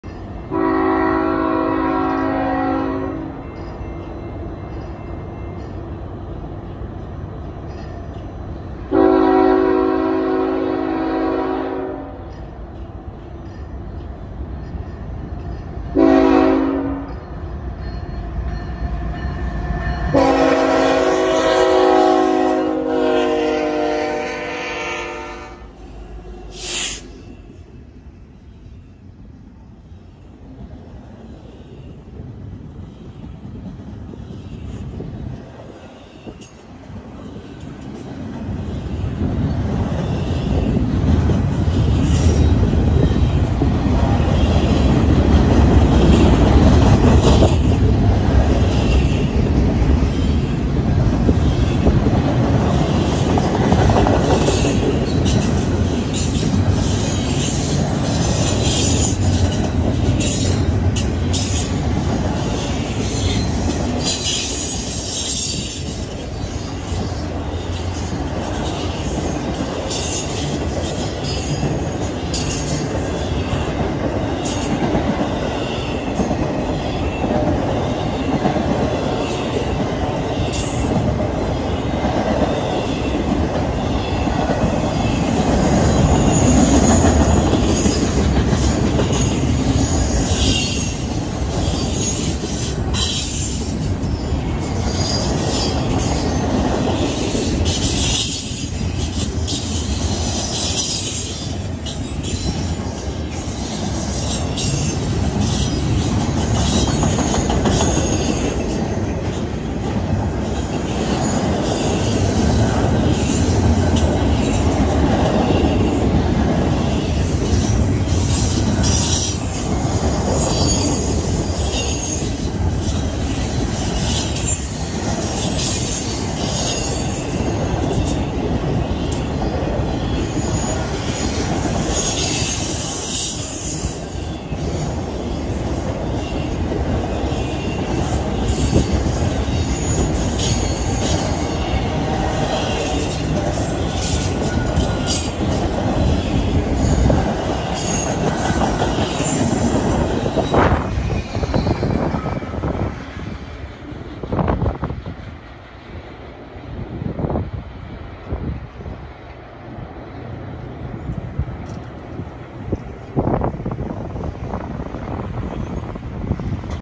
BNSF 3959 leads NS 28N in Charlotte with one of the best lashups! 2-6-2023
BNSF ET44C4 3959 leads a good size NS 28N in Charlotte with probably one of the best lashups I’ve ever seen!

Consist
BNSF 3959
KCS 4828
BNSF 8995

99 intermodal buckets (7,000 feet)